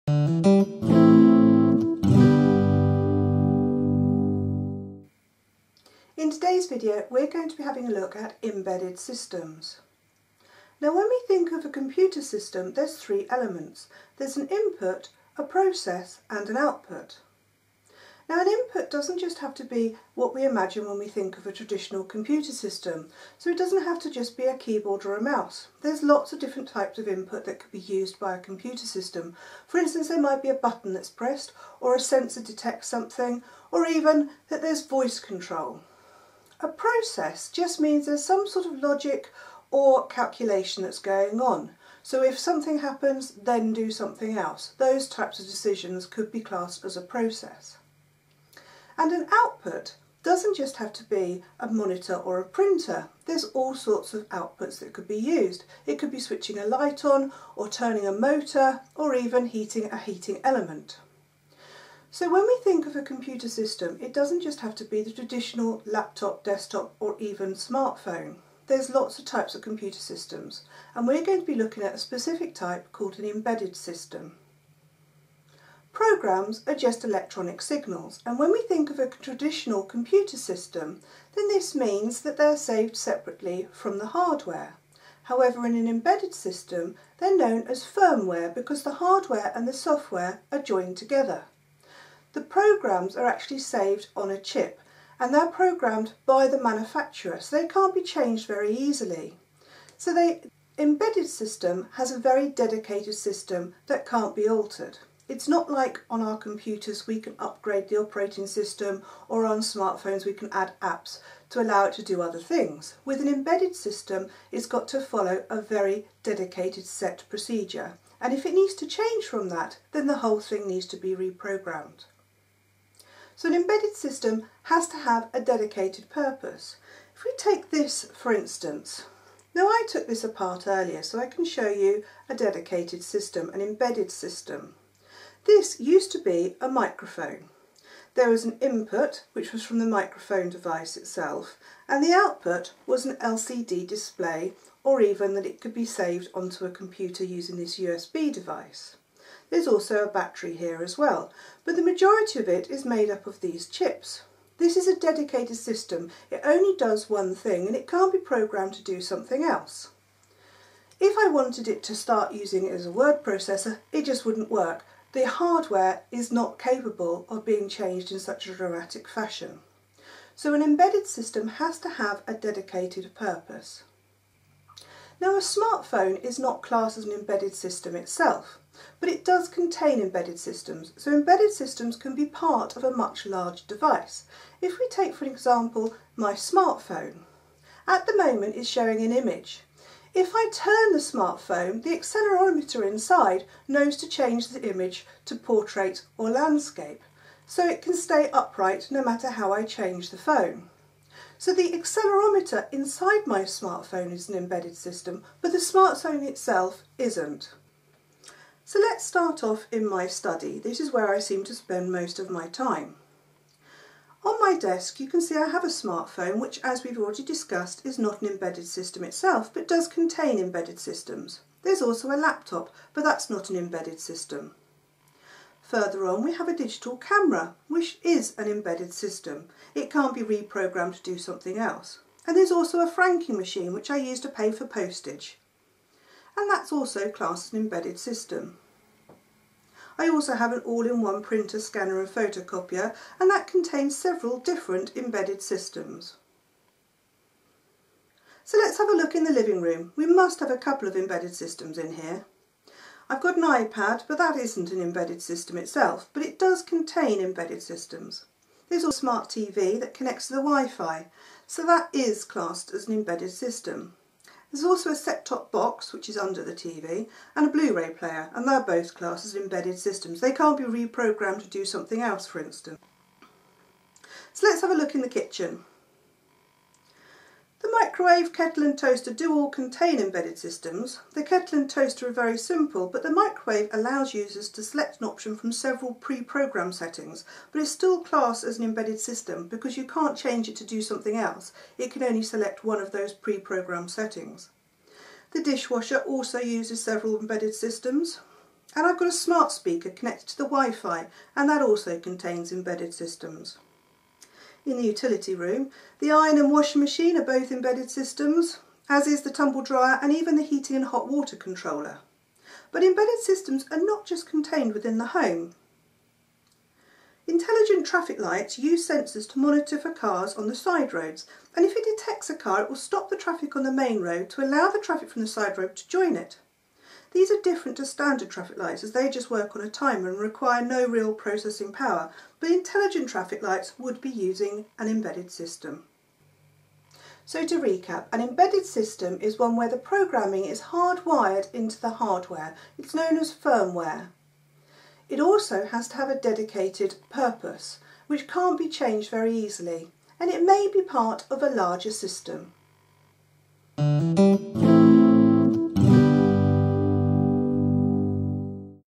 Embedded Systems
Ideal for GCSE Computer Science students.  Teaches what embedded systems are and gives examples.